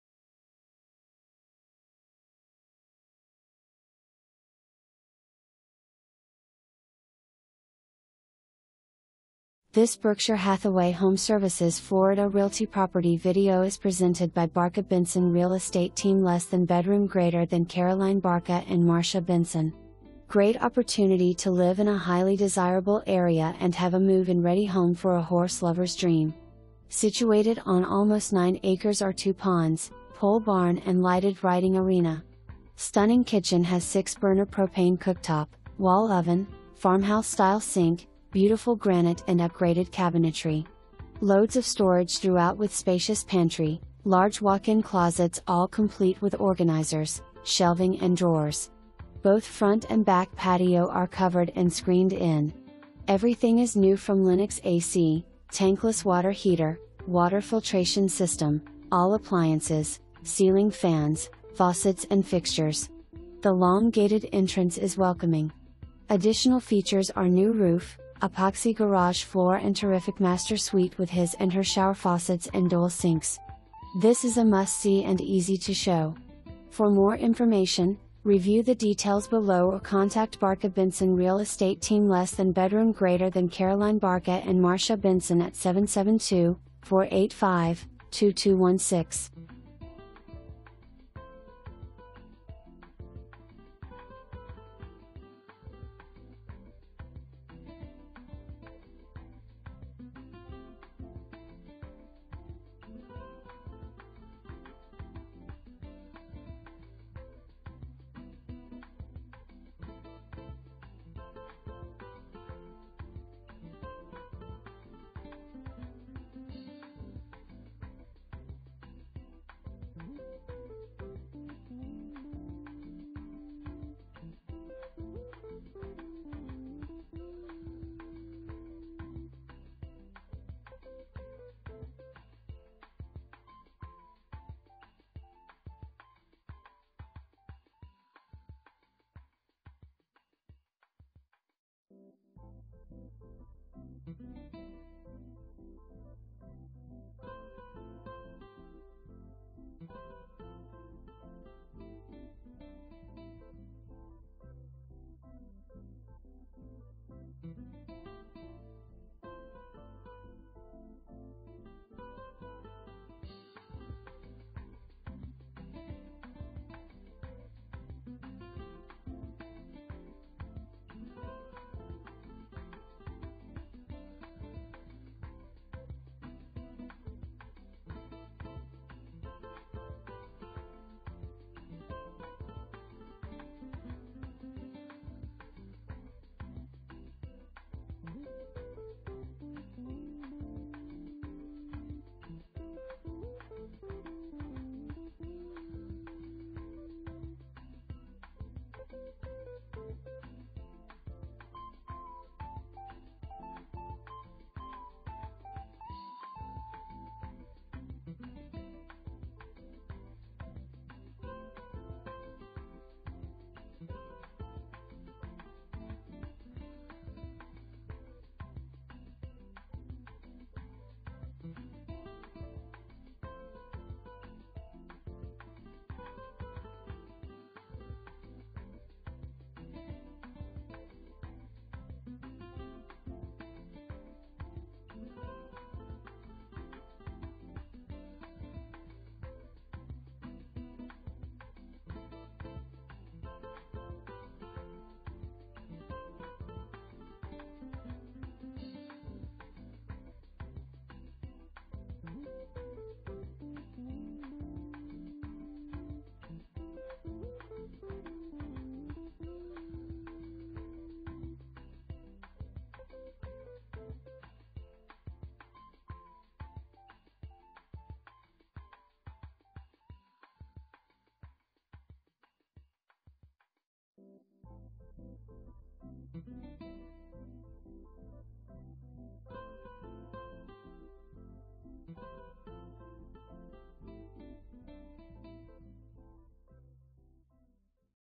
Berkshire Hathaway HomeServices Florida Realty - 2756 S Brocksmith Road
Great opportunity to live in a highly desirable area and have a move-in ready home for a horse lover's dream. Situated on almost 9 acres are 2 ponds, pole barn and lighted riding arena. Stunning kitchen has 6 burner propane cooktop, wall oven, farmhouse style sink, beautiful granite and upgraded cabinetry. Loads of storage throughout with spacious pantry, large walk-in closets all complete with organizers, shelving and drawers. Both front and back patio are covered and screened in. Everything is new from Lennox A/C, tankless water heater, water filtration system, all appliances, ceiling fans, faucets and fixtures. The long gated entrance is welcoming. Additional features are new roof, epoxy garage floor and terrific master suite with his and her shower faucets and dual sinks. This is a must see and easy to show.

Barca Benson Real Estate Team Caroline Barca & Marcia Benson